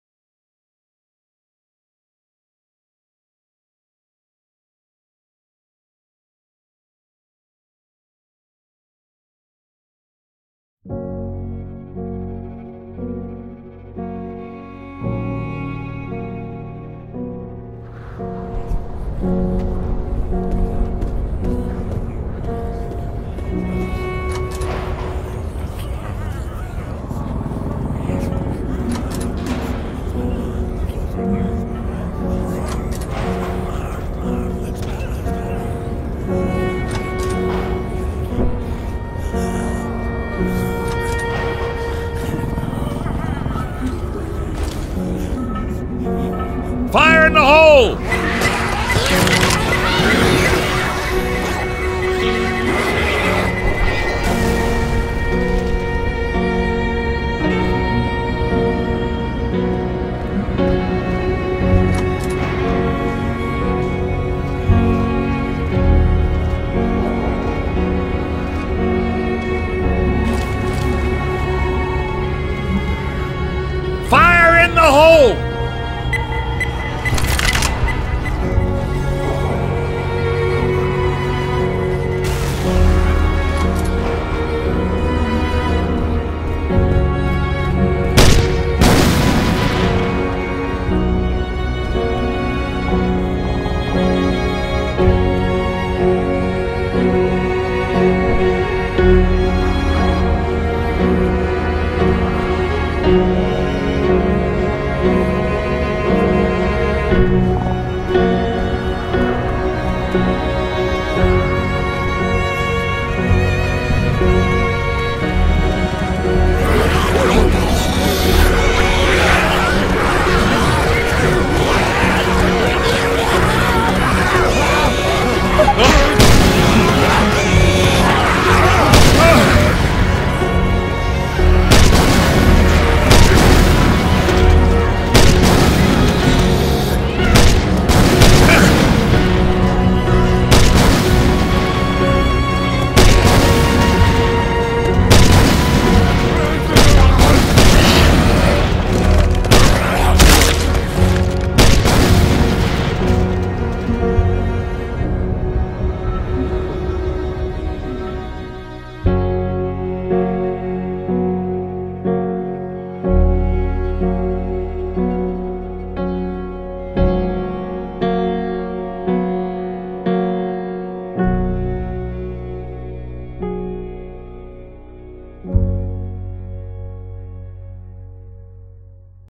Memories of my family
the song is not mines so here is the link to the actural song OK YOUTUBE DONT MUTE ME!!! anyways i felt like doin a memorial for the fallen so sit back and relax and enjoy the vid ^^